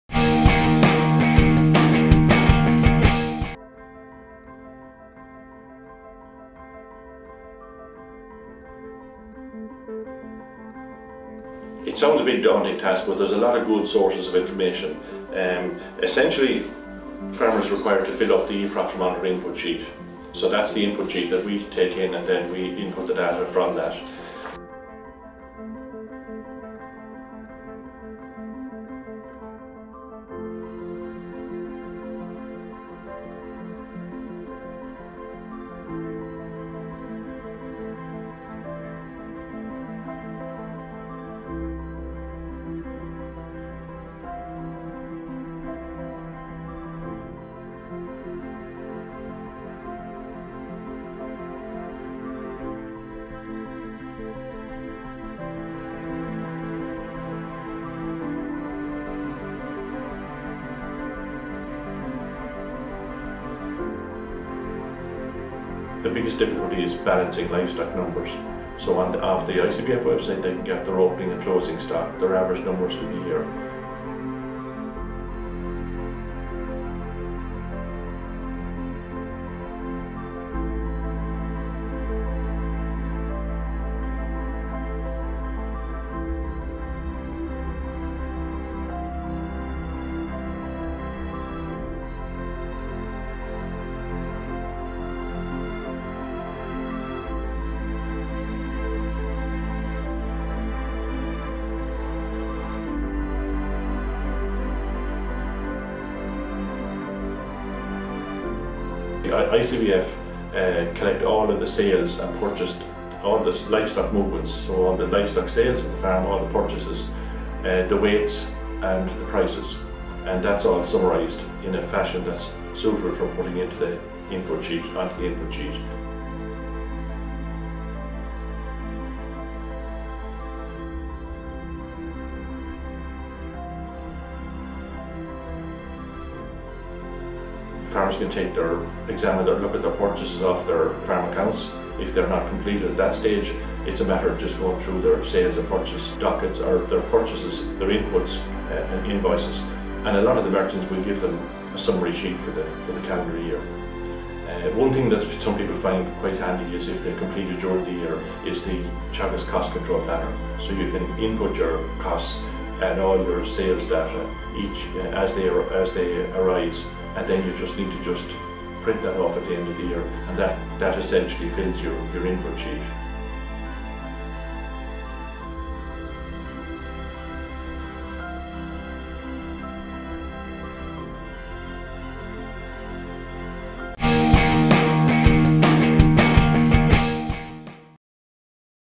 BETTER farm: how to do an e-Profit monitor
This video explains the necessary steps in order to complete your e_Profit monitor.